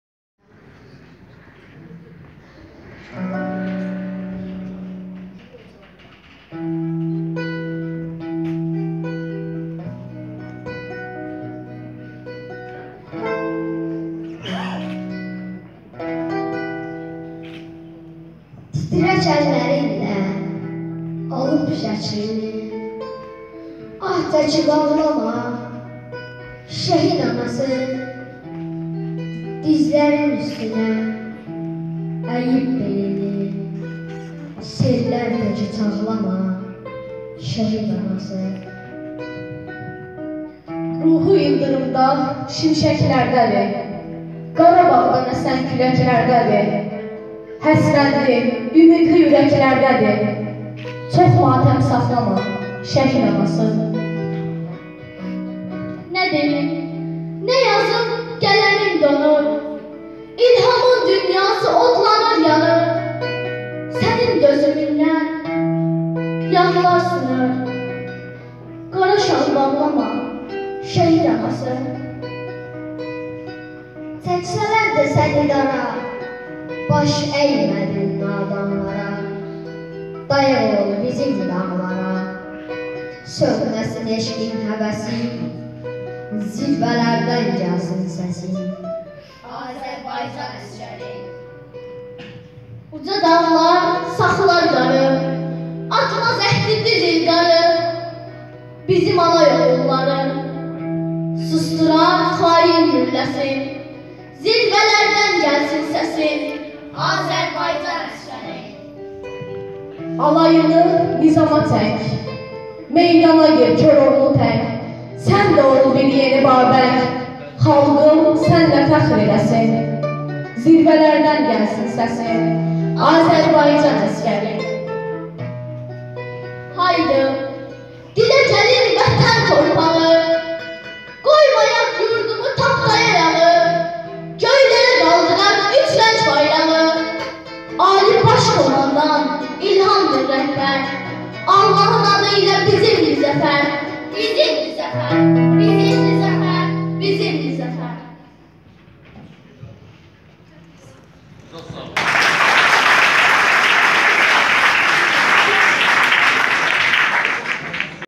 Semkir Kur medeniyyet Genc Heyderciler bedii qiraet derneyi "Şehid anasi"
Aprel şehidi Vusal Aliyevin anim tedbirinde cixiw.23.02.2017.